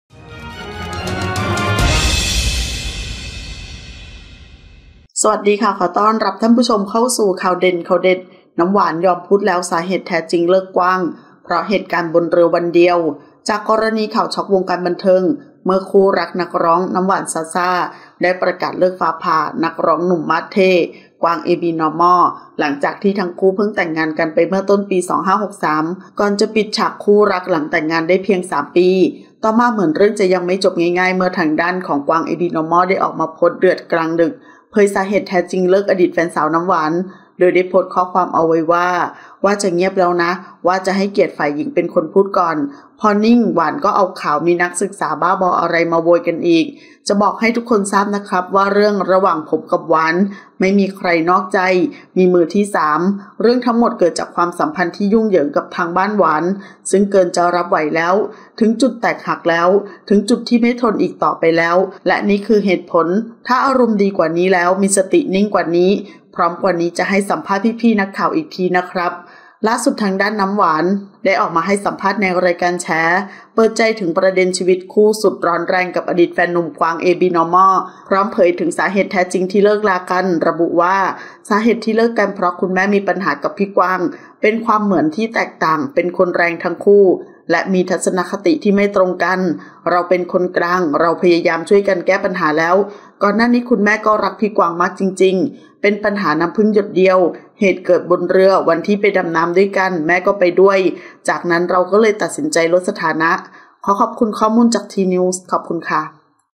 “น้ำหวาน” ยอมพูดแล้ว สาเหตุแท้จริงเลิก “กวาง” เพราะเหตุการณ์บนเรือวันเดียว
00:00 - Intro
00:10 - News
01:00 - Minute